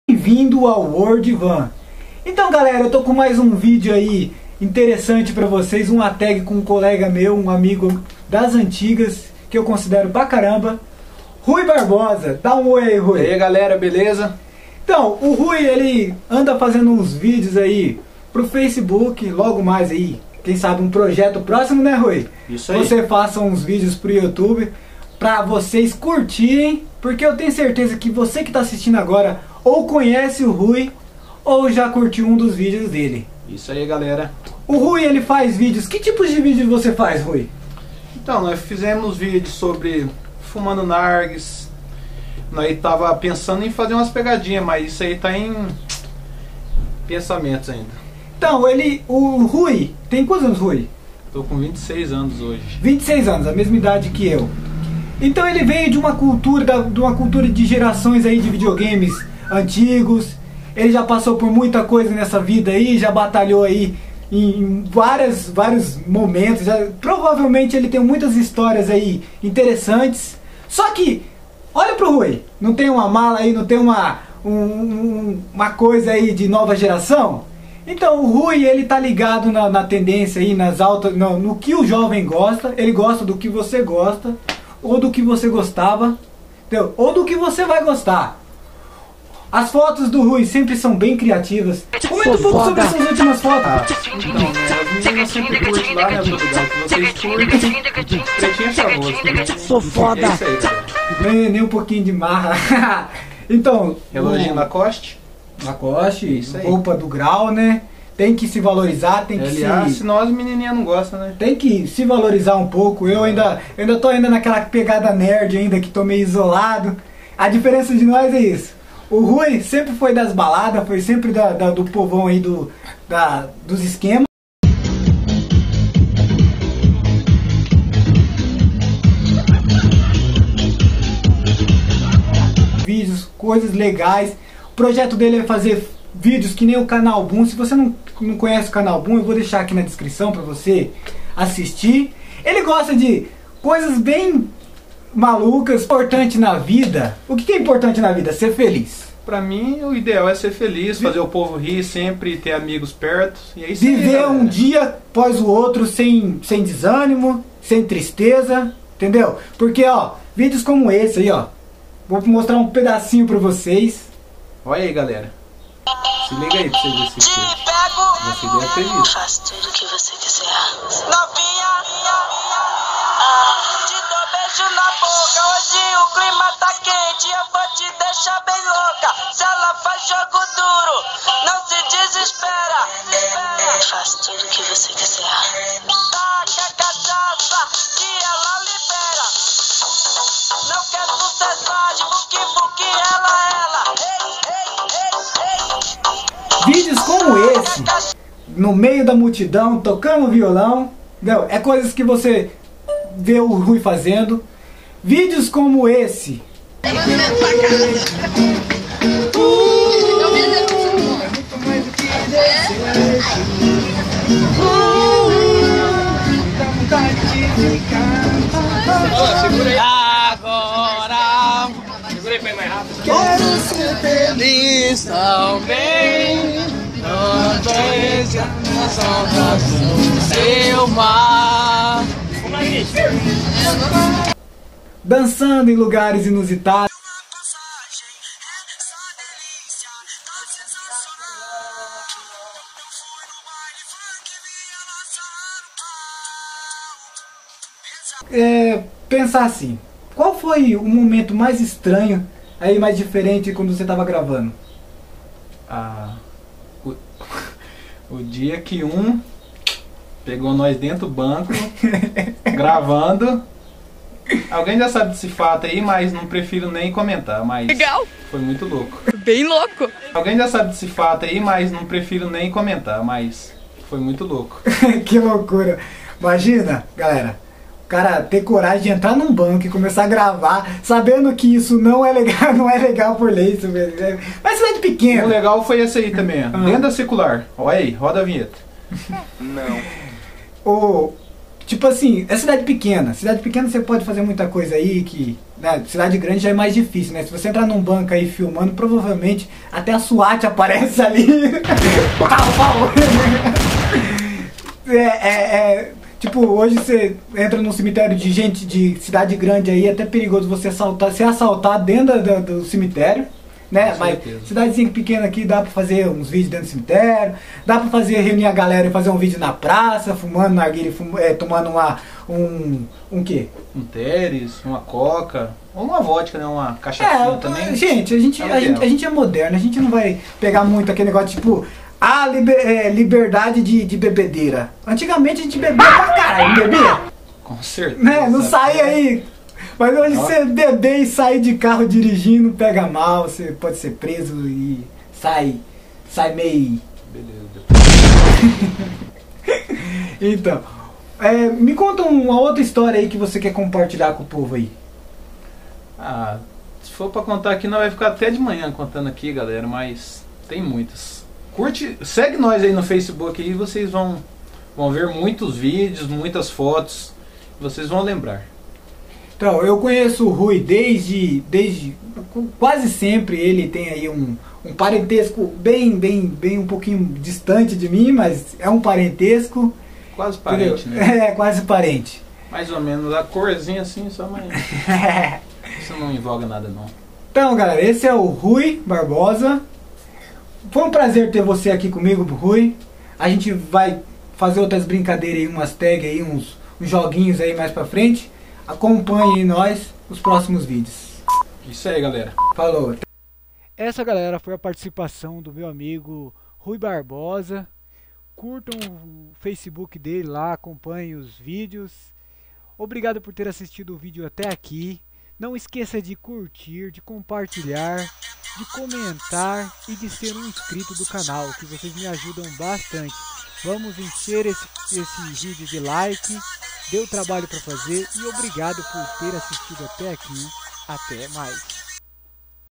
descontração e intervista ( dia com rui)
esse vídeo foi um papo sobre  videos postado no youtube e no face coisas legais que fazemos no dia dia  filmagens e alegria , com edição e algumas coisas para dar risada e refletir... espero que se divirta